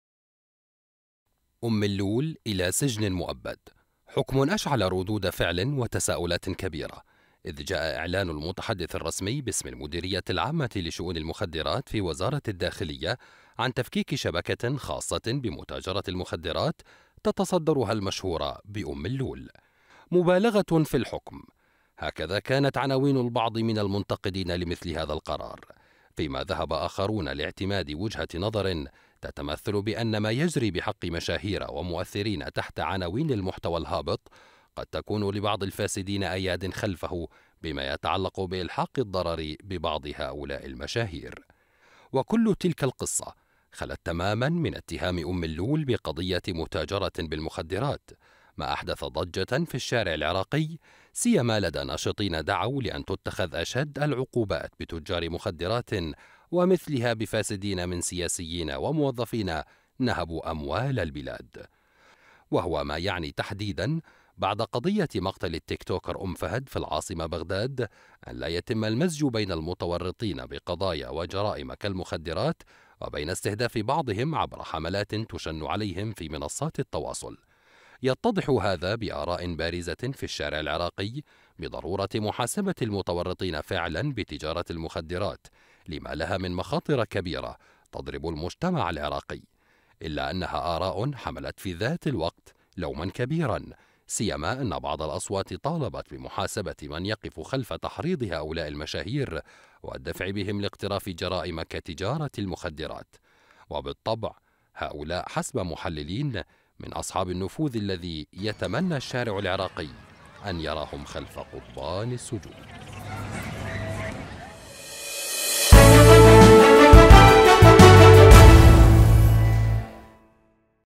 الحكم على ام اللول بالسجن المؤبد يثير جدلا واسعا في العراق | تقرير
موقع قناة التغيير الفضائية  الالكتروني
ام اللول سجن مؤبد